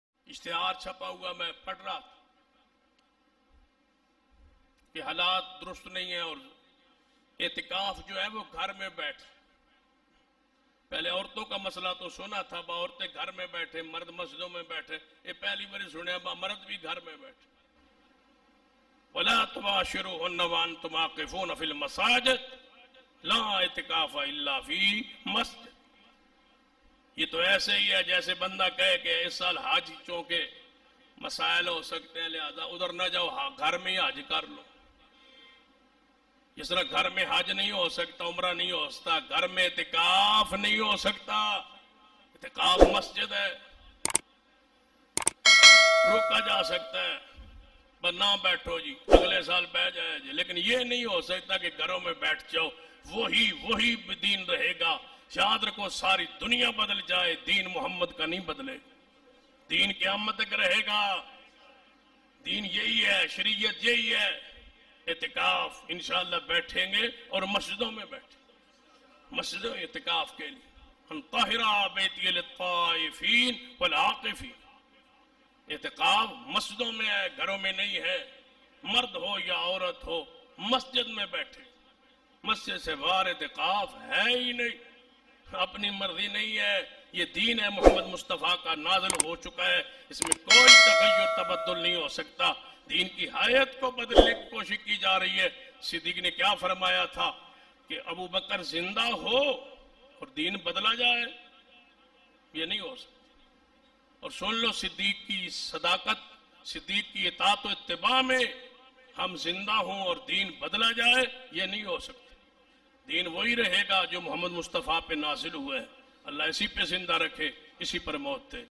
Halaat ko dekh kar kya Ghar mein Ihtikaf ho sakta hai By Syed Sabtain shah naqvi
Roza ke Masail By Molana Syed Sabtain Shah naqvi